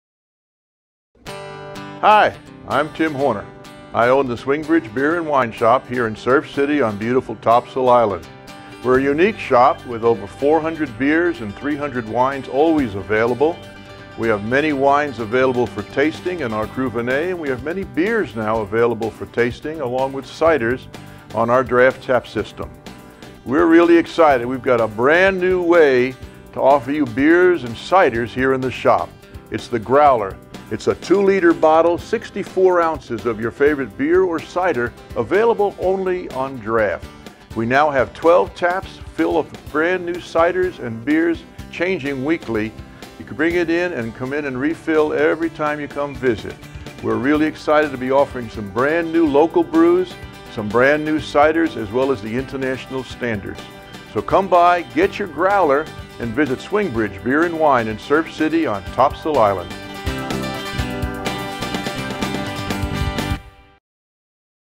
SwingBridge Beer And Wine - The Growler
Tim Horner owner of SwingBridge Beer And Wine in Surf City, NC gives an overview of the Topsail Island store and talks about the Growler a refillable bottle of beer or cider.